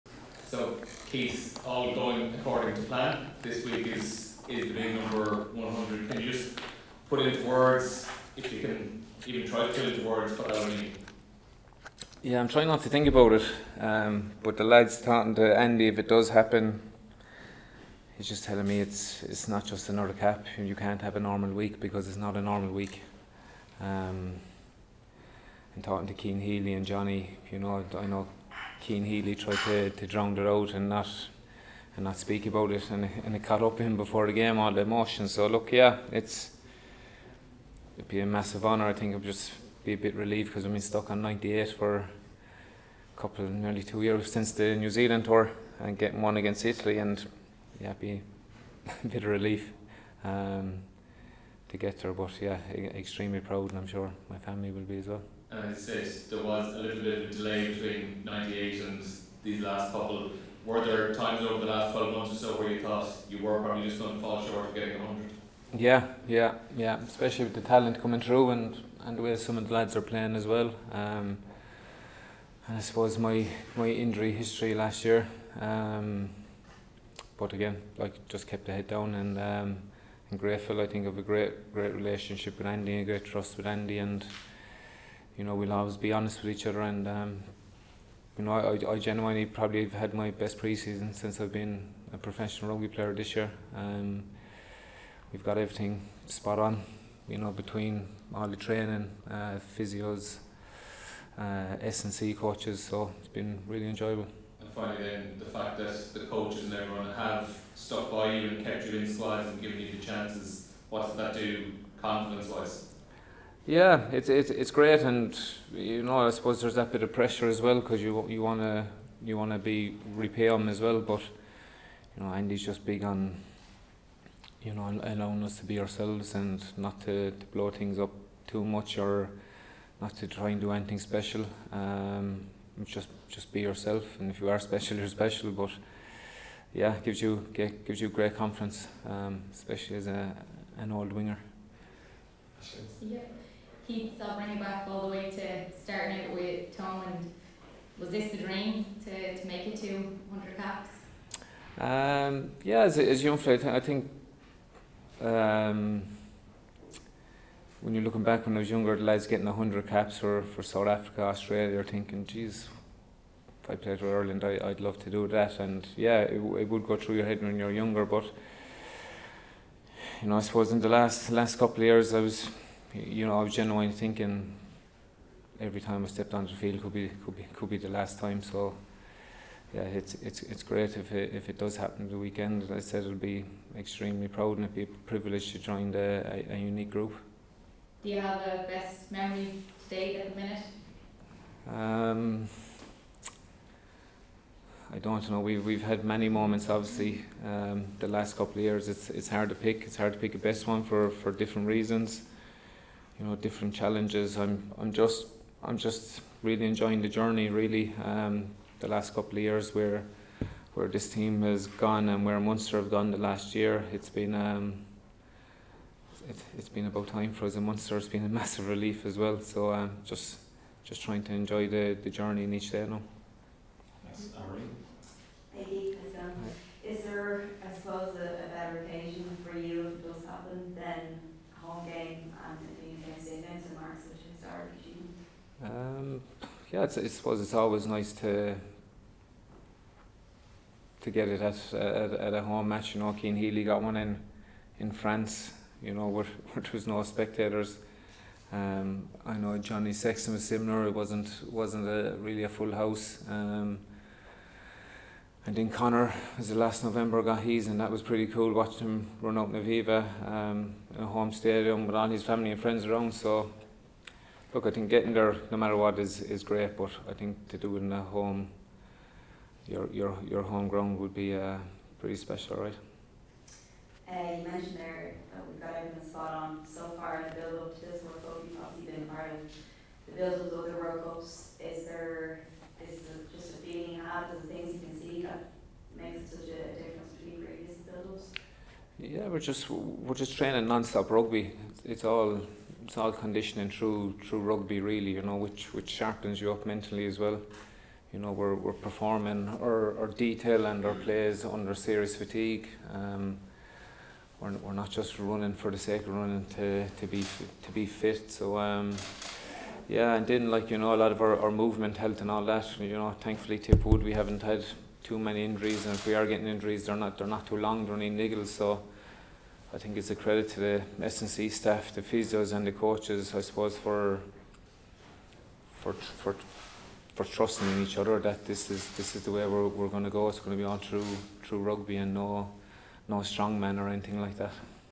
The journey to 100 Ireland caps | 'Something I never thought would happen' | KEITH EARLS
Fresh from their camp in Portugal, soon-to-be Ireland centurion Keith Earls addressed the media in the build up to their World Cup warm-up game against England on Saturday.

Rugby on Off The Ball with Vodafone

- Snapchat: add us and message the show @offtheball1